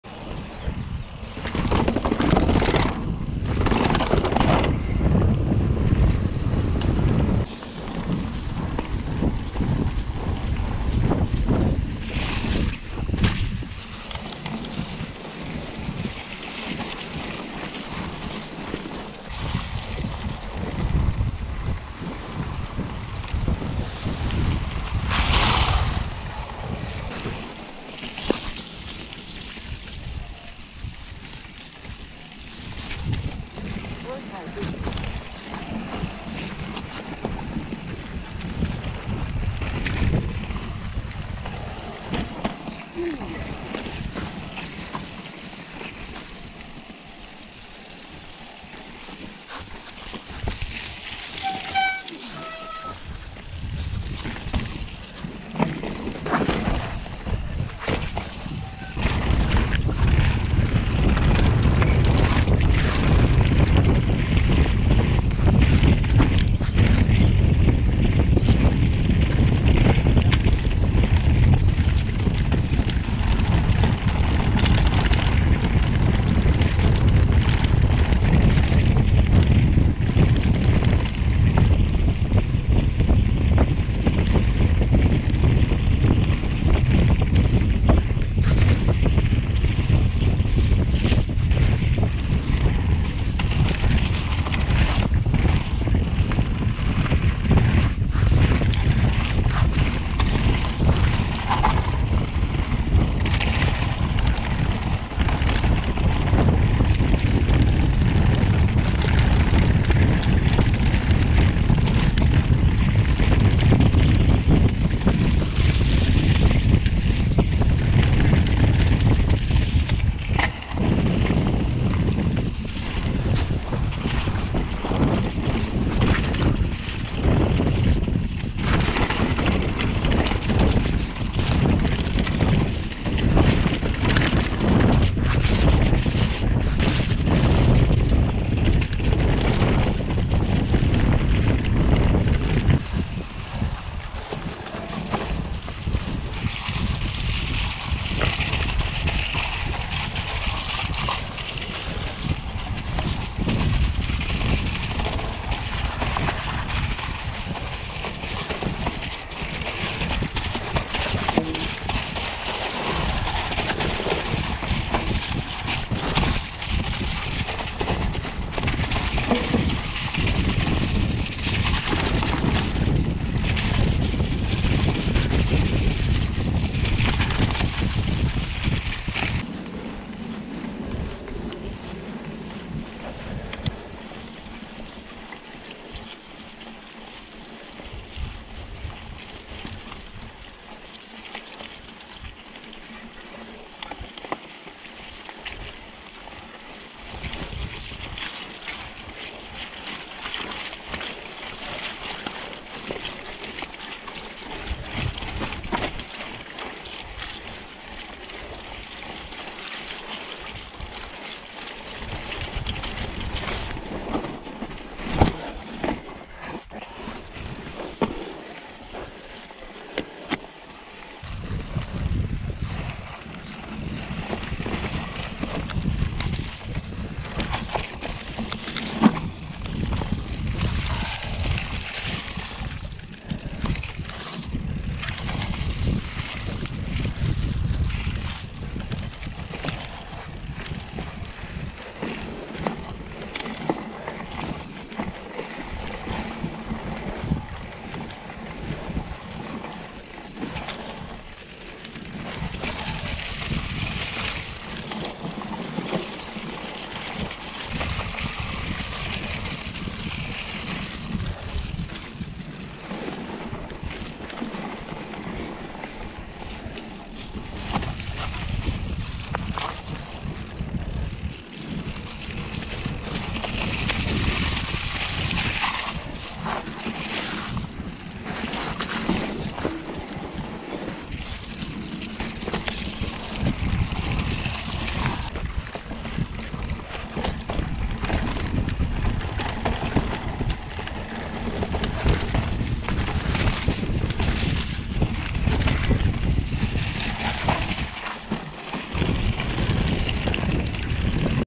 MTB 221219 Peel twr etc
Another bit of a mudfest with Ste, Tony, Marc, Ian & oscar.